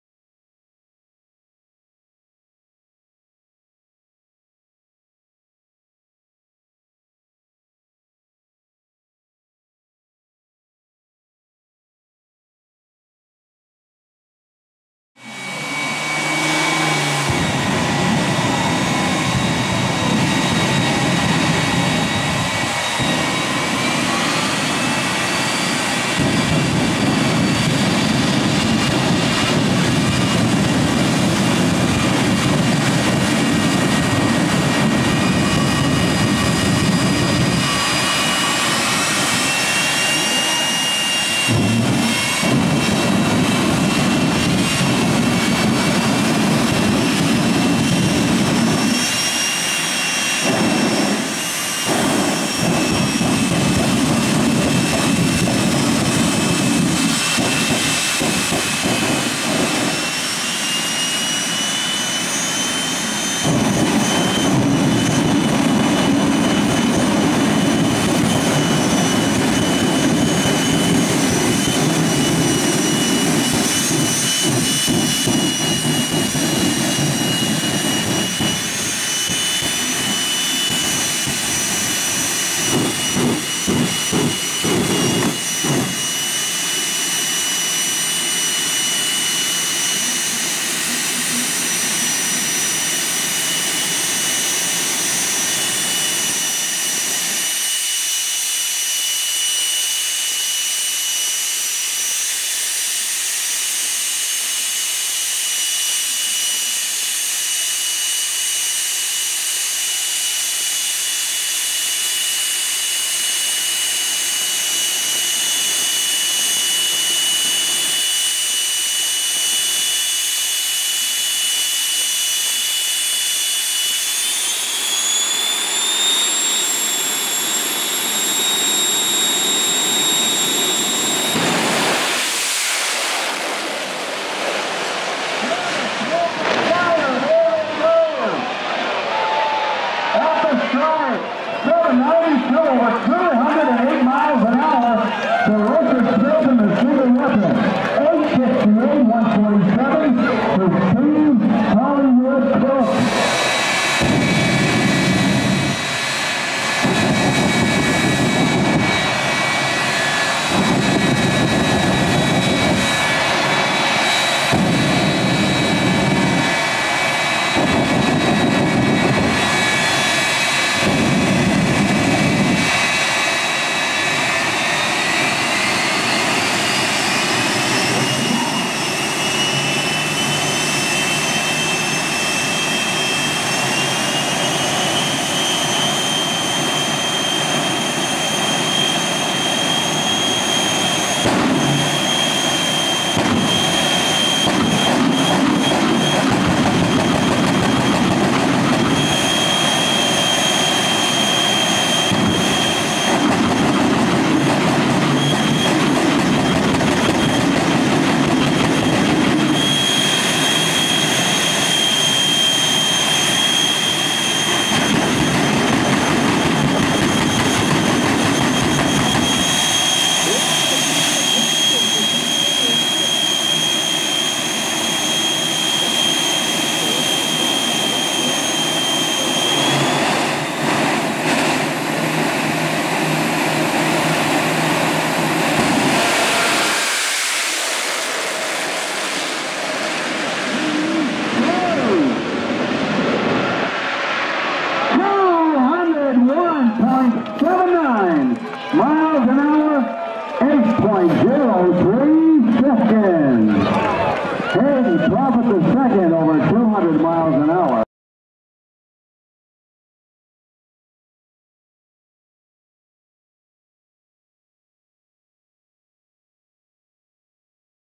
2016 IHRA Rocky Mountain Nationals Part 25: (Jet Car Exhibition)
Jets on a saturday night!

and like me on facebook!